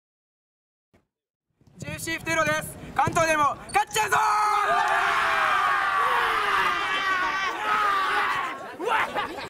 ⚽JFC FUTURO（2019高円宮杯U-15関東大会／神奈川県第２代表）
▽神奈川県第２代表：JFC FUTURO Jr.ユース

【2019年度 高円宮杯 JFA 第31回 全日本ユース（U-15）サッカー選手権大会・関東予選】

・大会期間：2019年11月9日（土）～17日（日）※関東・各都県会場